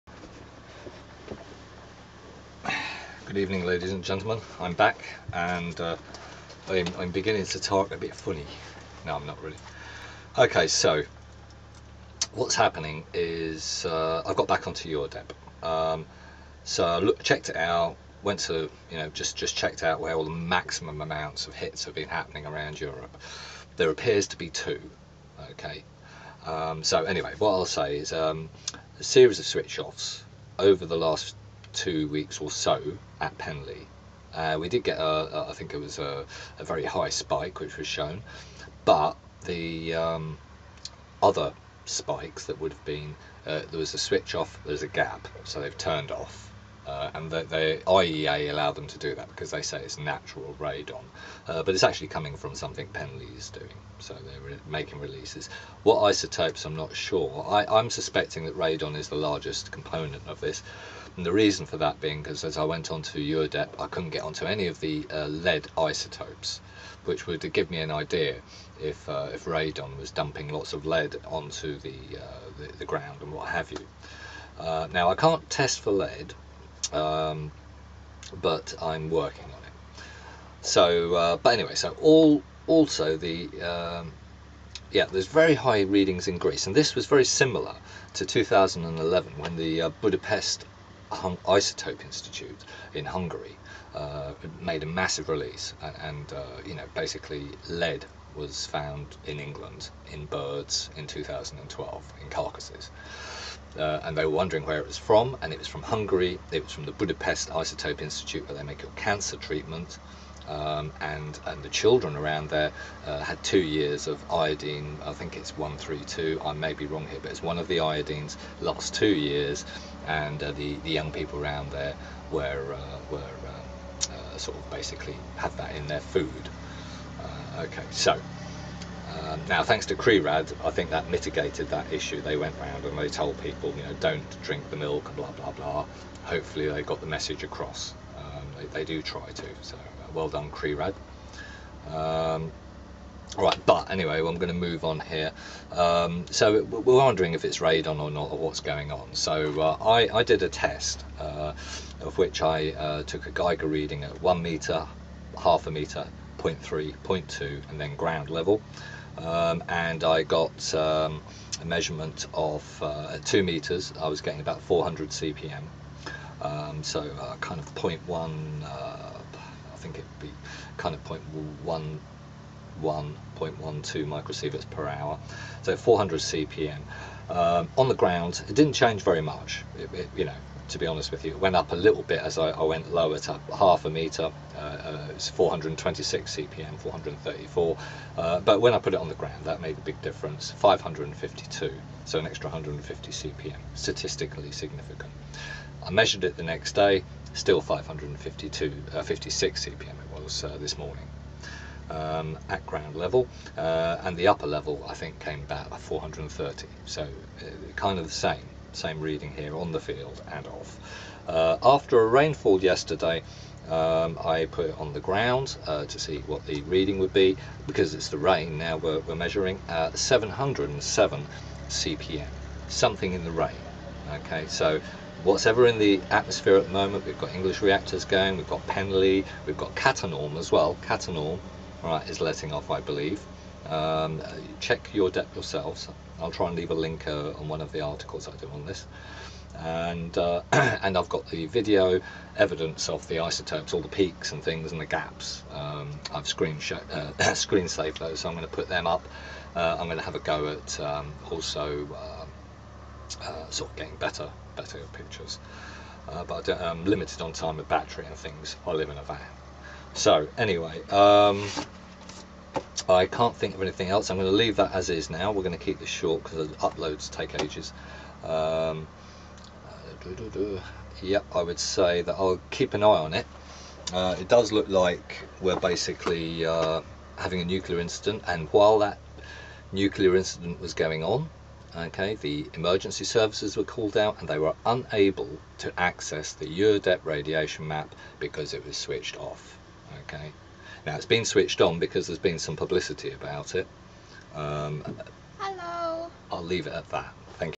Nuclear releases over Europe - Penly, Catternom and the Budapest Medical Isotope Institute?
EURDEP is back on and it is showing VERY high peakd in Greece and moderately high peaks in the north western sections of Europe.. Multiple hits over the past few weeks.. And all covered up by EURdEP Germany? (I stand to be corrected.) And the Italian Eurdep has switched it back on (as usual..

Greece showing a MASSIVE spike as does some surrounding countries monitoring sites including Austria. I cant rule out Turkey but the winds can travel from hungary to greece via the coastal medittereanian route like it did in 2011 after the Hungarian Budapeast Medical Isotope Institutes HUGE releases just AFTER the Fukushima Daichi triple meltdowns. The IAEA even tried to blame the Japanese instead of the Hungarians the medical disaster.. The IAEA is respponsible for that reactor anyway.. Go figure.

Plenty of monitoring "gaps" at the Penly radiation monitoring stations..

Recent strikes can be seen too! Last time i checked yesterday night..

Watch the rain as we dont exactly know whats in it..  There could be many isotopes but my initial findings point to largely Radon.. which could be hiding other alpha and  beta energy isotopes.. concerning the uk

3 and a half hours to upload this video :(  blimey..  i better put the kettle on then.. :)